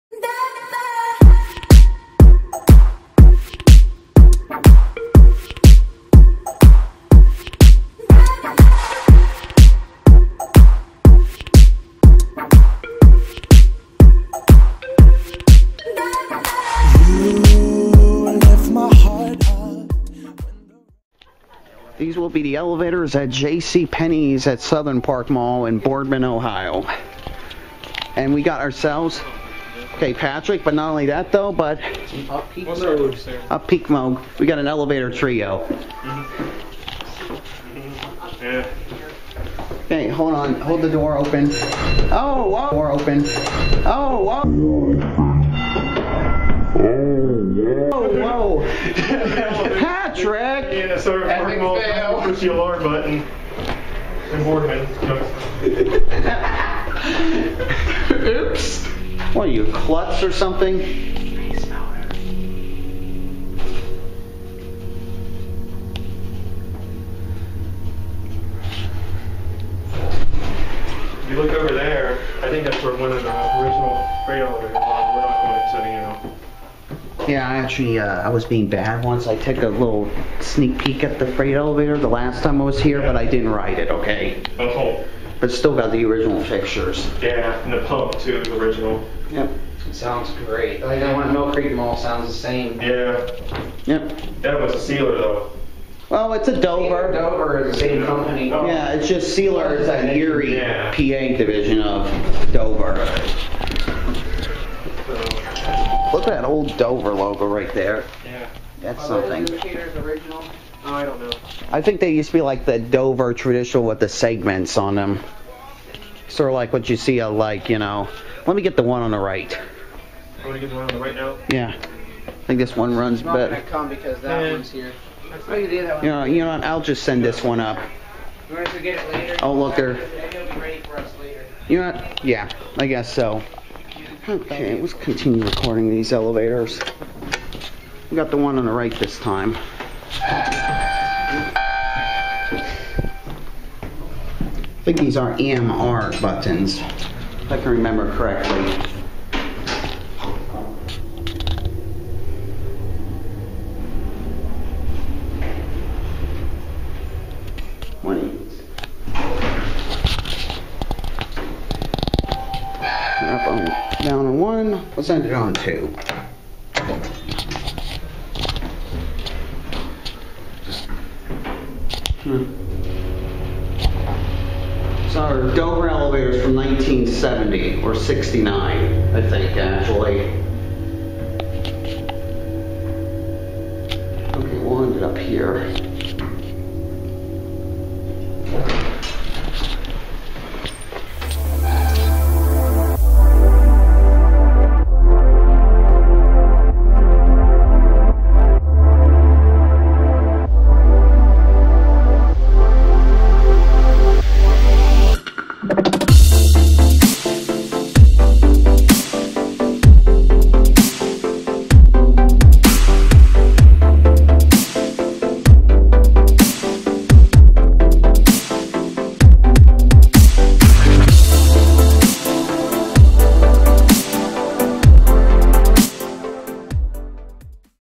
DOVER Hydraulic Elevators in JCPenney at Southern Park Mall, Boardman, OH (Featuring ALARM FAIL)
[Recorded with Patrick Nedz and Ryan Rediy (AKA YouTuber Toronto Elevators)]

This was a pretty interesting trip to Southern Park Mall. Not only it was the first time I met Ryan Rediy (AKA YouTuber Toronto Elevators) but I've actually met two nice technicians from KONE -- who happened to be working on the escalators in JCPenney at the time... BTW -- that video will be out soon. Also Patrick accidently hit the alarm button instead of the door open one! LOL At least it wasn't connected to the phone; if that was the case -- then this would have been an EPIC FAIL!